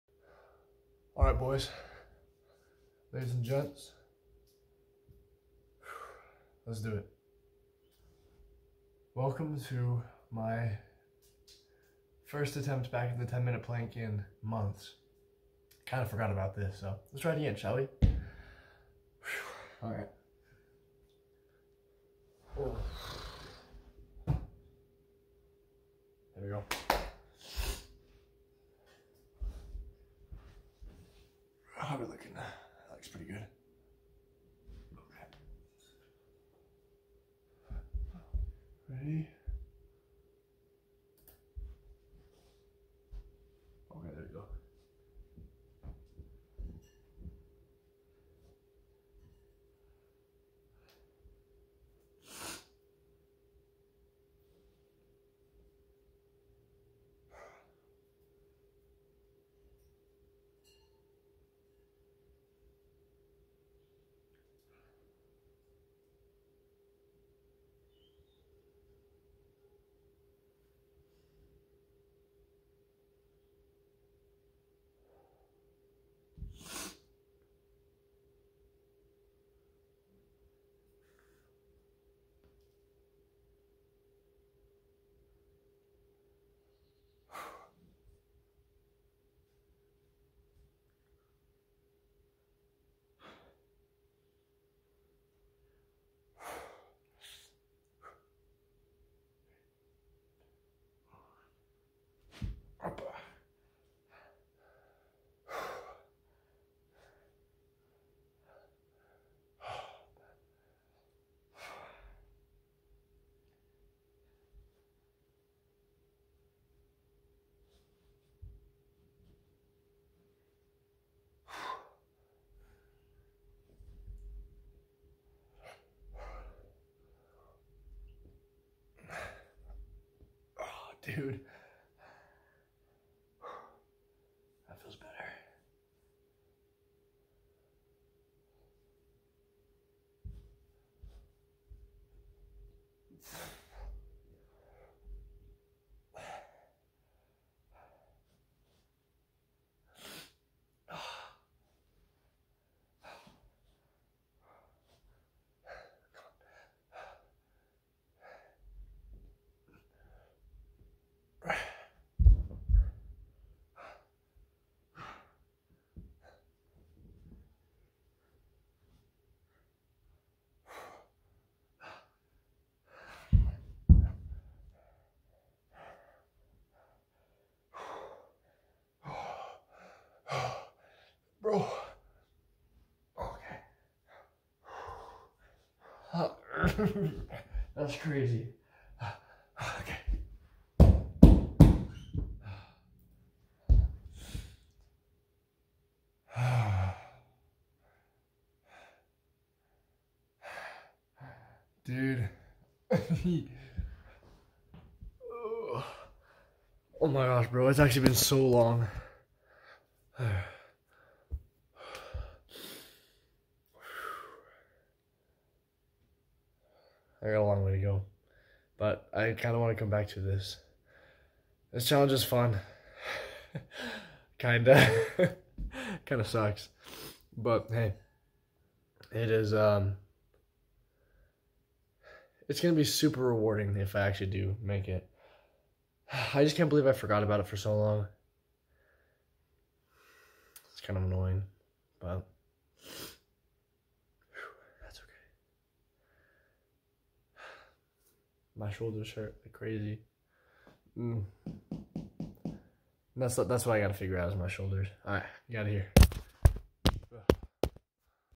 10 Minute Plank Attempt 8 Foo
It's been months since I last attempted this, so let's get back on the horse.